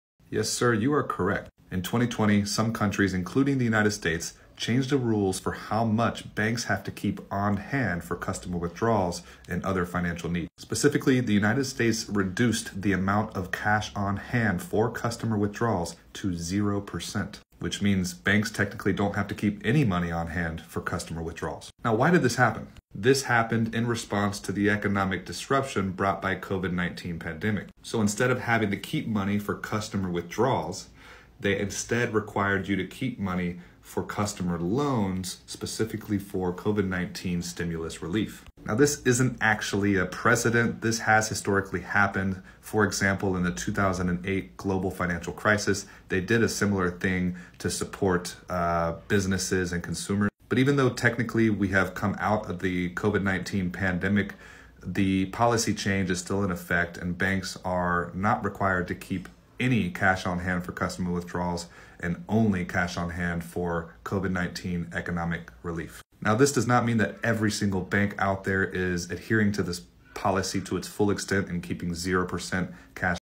How COVID-19 Changed Banking Forever: The Shocking Truth About 0% Reserve Requirements
Join Other Aspiring Micro-Entrepreneurs Assembling to Learn & Earn in a